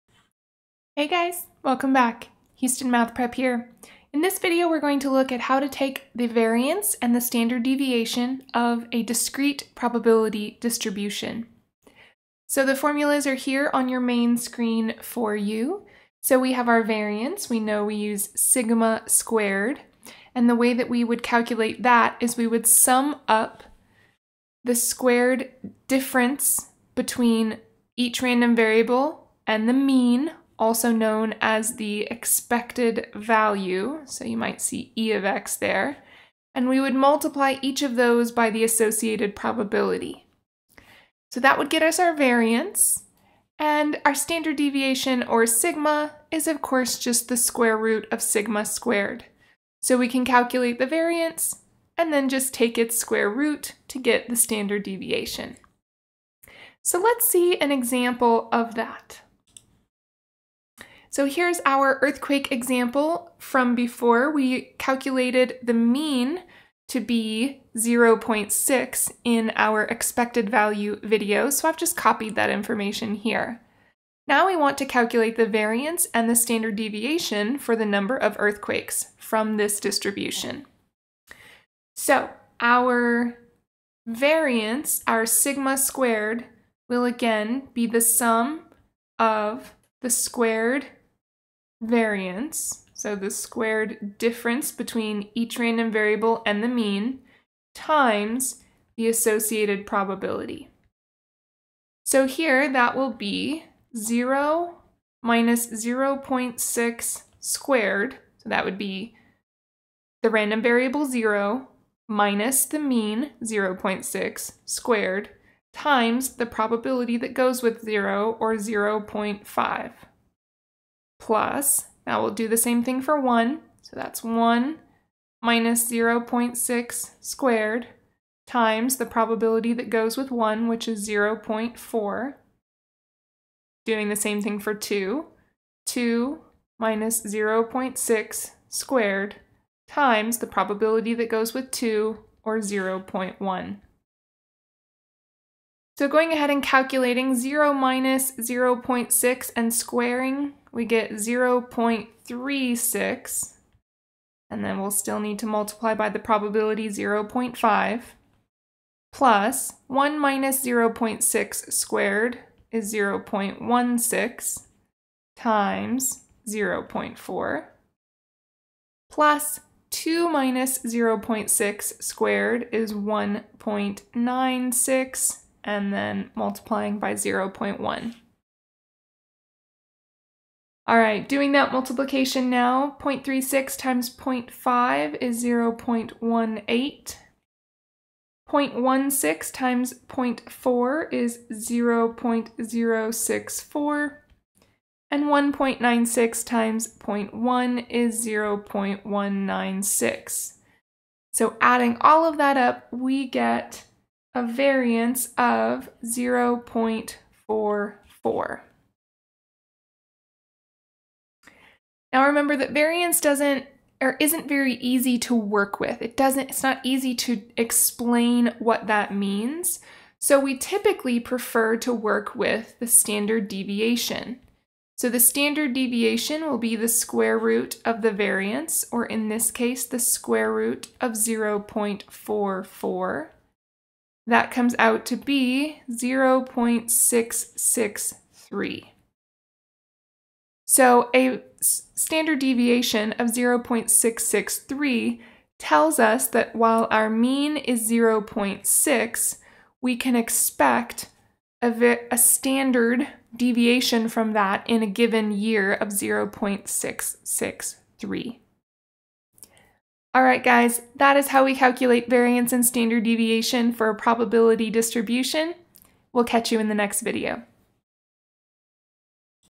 Variance & Standard Deviation of Discrete Distributions
How to calculate the variance and standard deviation of a discrete probability distribution